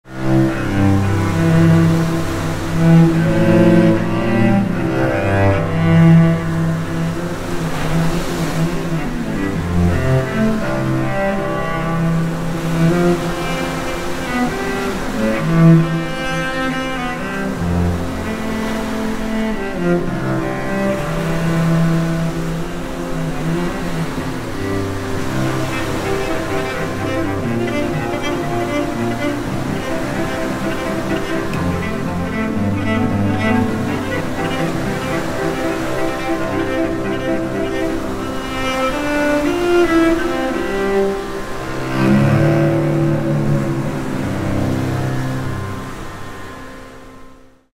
Little Raga Session 45
“Little Raga Session 45” Waves & My Main Instrument 165 years old Cello from Germany 🇩🇪 staystrong Please Use 🎧 Minute long improvisation everyday on different instrument. stayhome littleraga mazārāga songsofcomfort togetherathome paliecmājās silkroadgmw 🎥 my eight years old Elza quarantine stayhome cello latvia germany🇩🇪